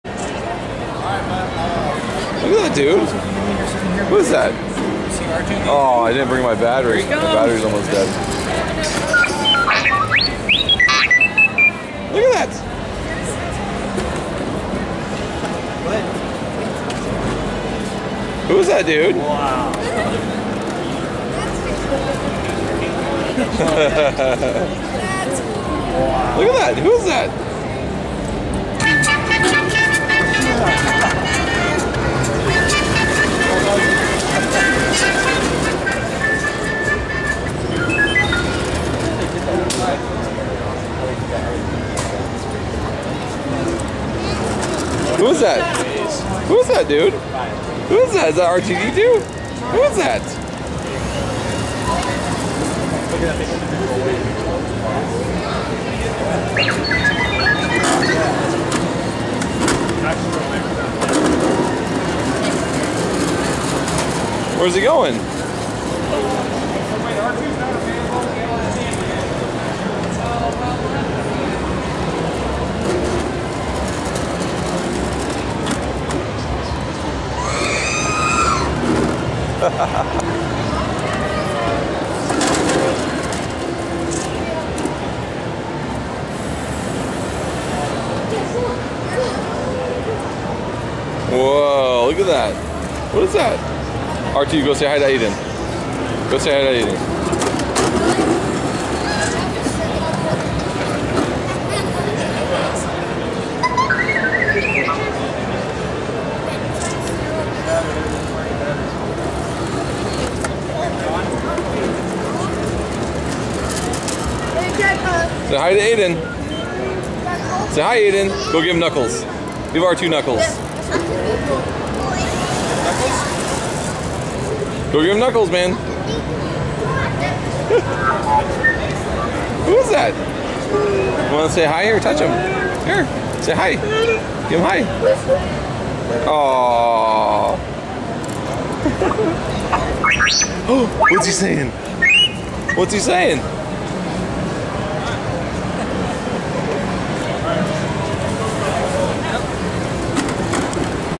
My 2 year old meeting R2D2 for his first time!
@ Comic-Con Phoenix 2011 where my 2 year old son met Star Wars R2D2 for the very first time!